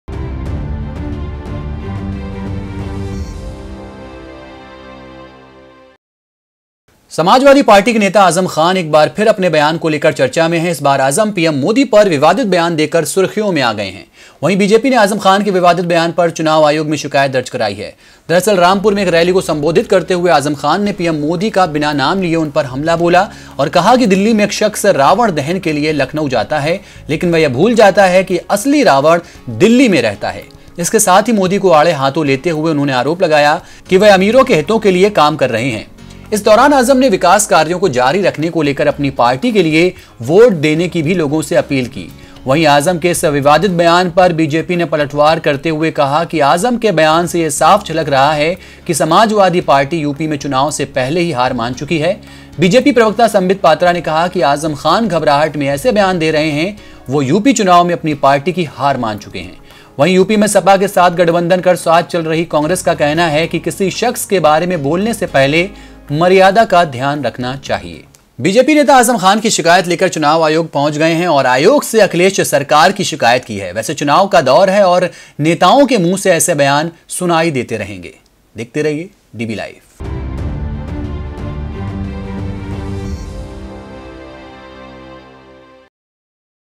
DB LIVE | 6 FEB 2017 | up election 2017 azam khan attacks pm narendra modi
समाजवादी पार्टी के नेता आजम खान एक बार फिर अपने बयान को लेकर चर्चा में हैं। इस बार आजम पीएम मोदी पर विवादित बयान देकर सुर्खियों में आ गए हैं। वहीं बीजेपी ने आजम खान के विवादित बयान पर चुनाव आयोग में शिकायत दर्ज कराई है। दरअसल  रामपुर में एक रैली को संबोधित करते हुए आजम खान ने पीएम मोदी का बिना नाम लिए उन पर हमला बोला और कहा कि दिल्ली में एक शख्स रावण दहन के लिए लखनऊ जाता है, लेकिन वह यह भूल जाता है कि असली रावण दिल्ली में ही रहता है। इसके साथ ही मोदी को आड़े हाथो लेते हुए आरोप लगाया कि वह अमीरों के हितों के लिए काम कर रहे हैं। इस दौरान आज़म खान ने विकास कार्यों को जारी रखने को लेकर अपनी पार्टी के लिए वोट देने की भी लोगों से अपील की। वहीं आजम के इस विवादित बयान पर बीजेपी ने पलटवार करते हुए कहा है कि आजम के बयान से ये साफ झलक रहा है कि समाजवादी पार्टी यूपी में चुनाव से पहले ही हार मान चुकी है। बीजेपी प्रवक्ता संबित पात्रा ने कहा कि आजम खान घबराहट में ऐसे बयान दे रहे हैं...वो यूपी चुनाव में अपनी पार्टी की हार मान चुके हैं। वहीं, यूपी में सपा के साथ गठबंधन कर साथ चल रही कांग्रेस का कहना है कि किसी शख्स के बारे में बोलने से पहले मर्यादा का ध्यान रखना चाहिए।तो बीजेपी नेता आजम खान की शिकायत लेकर चुनाव आयोग पहुंचे औऱ आयोग से अखिलेश सरकार की शिकायत की है...वैसे चुनाव है दौर है और नेताओं के मुंह  ऐसे बयान सुनाई देते रहेंगे।